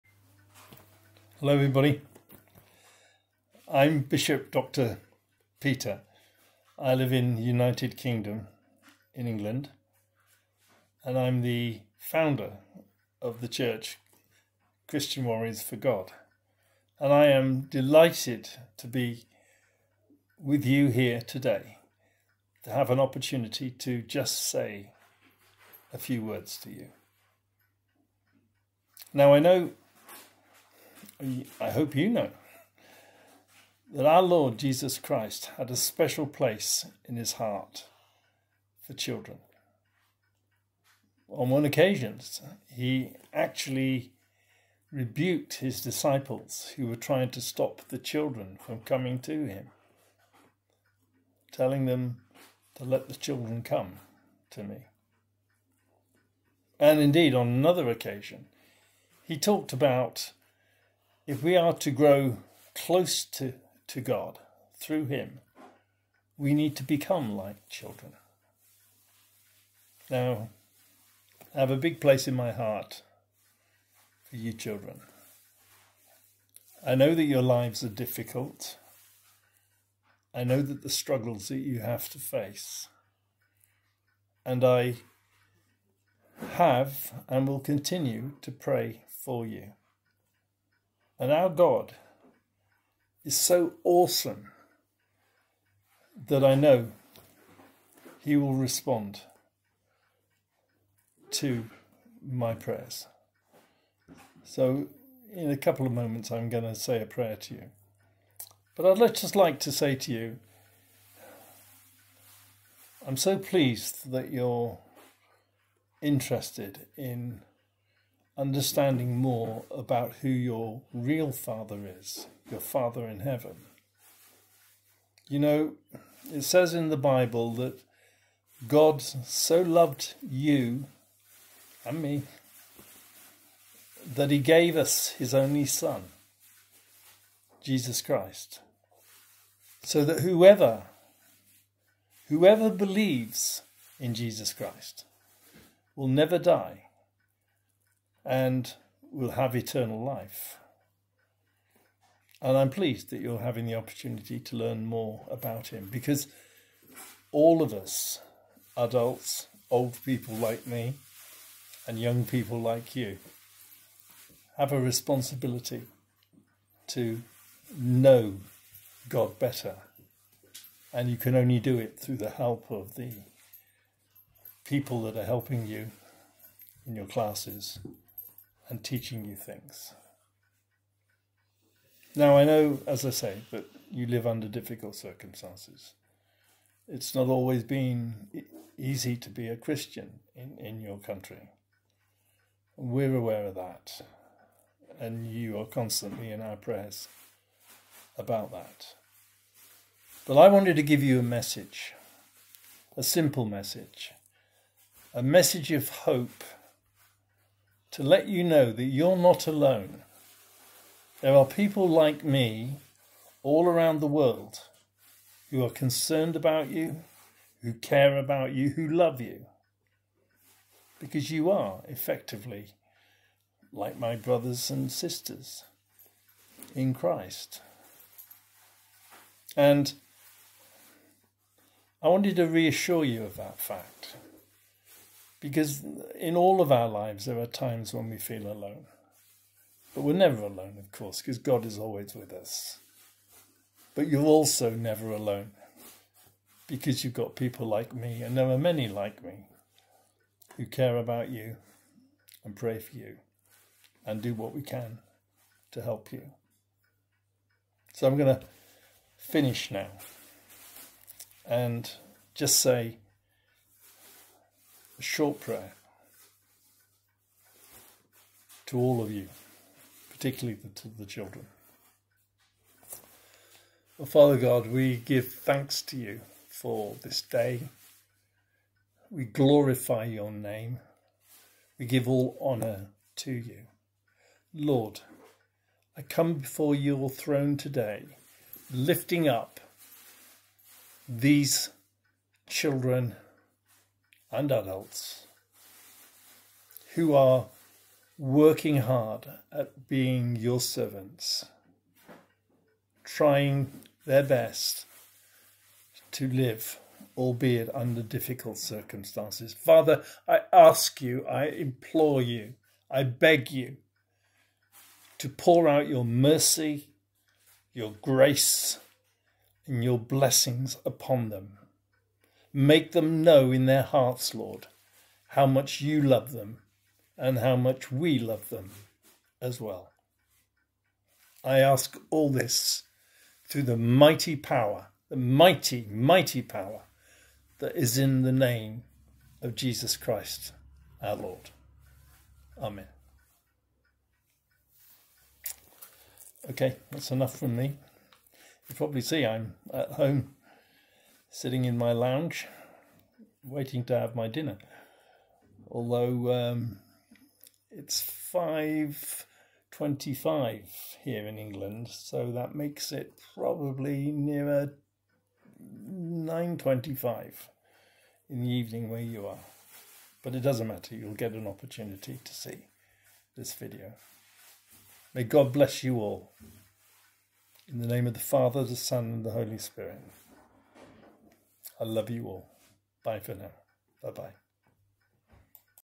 WELCOME MY YOUNG FRIENDS
Bishop Dr  Peter talks to young Christians about God love for them and reassures them that they are also loved and cared for by other Christians